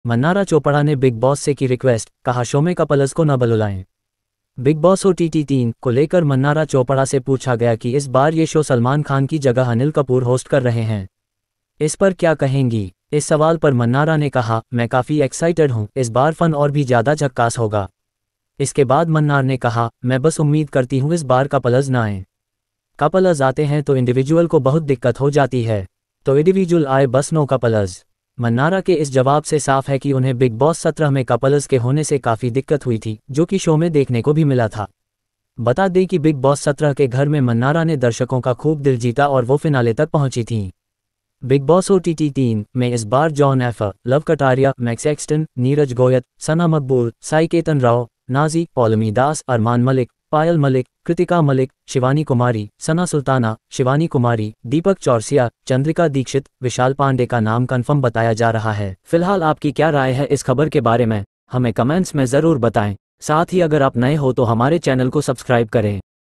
मन्नारा चोपड़ा ने बिग बॉस से की REQUEST 🙏
मन्नारा चोपड़ा ने बिग बॉस से की रिक्वेस्ट, कहा- शो में कपल्स को न बलुलाएं, क्योंकि उनकी वजह से...